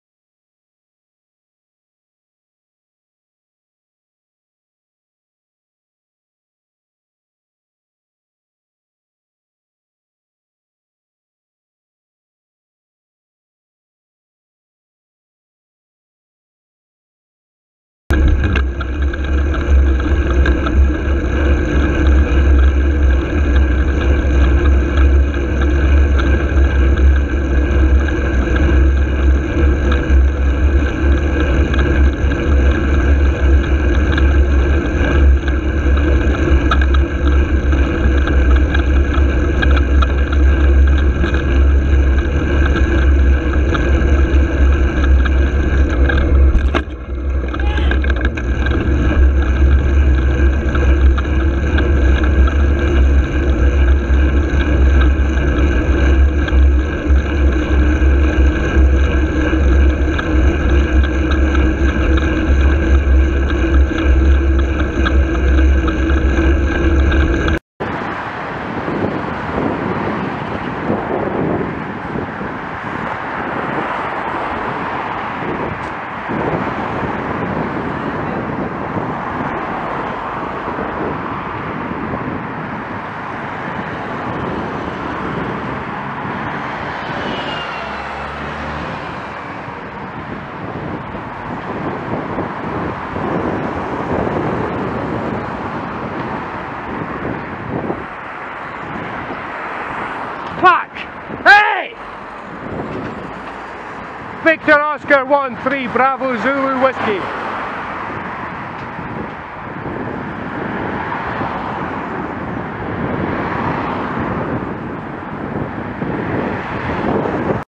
VO13BZW close pass at pinch point
Location: Brandon Road ( A428 ) beside the junction for Bredon Avenue.

Wednesday 24th May 2017 07:47